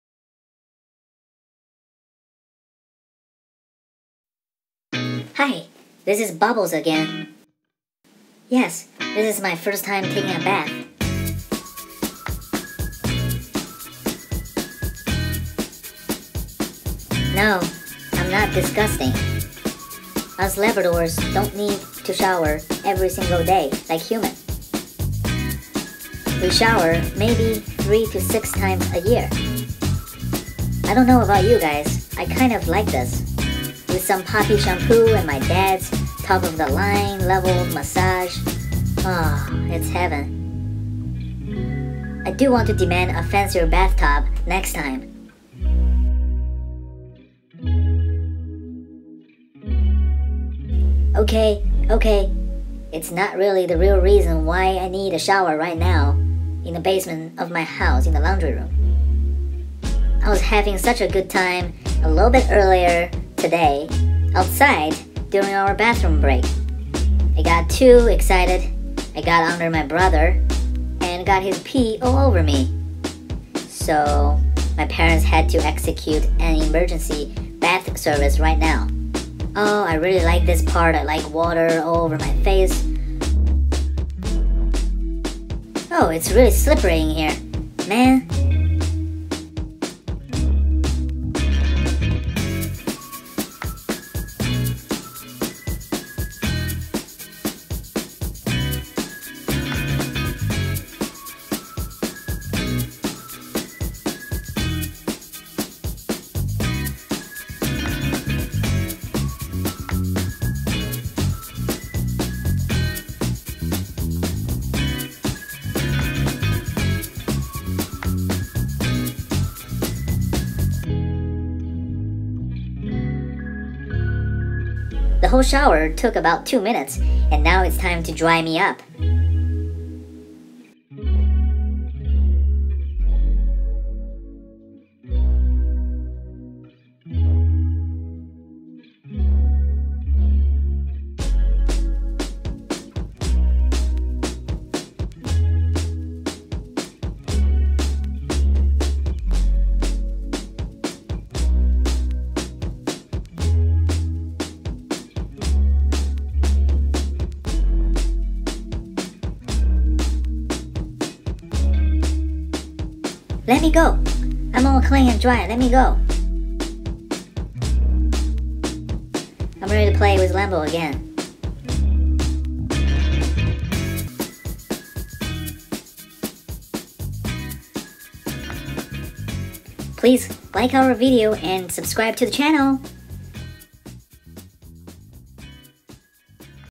Labrador puppy first bath-Bubbles First Times
Our Labrador puppy took his first bath! Would he hate it or love it, we will find out!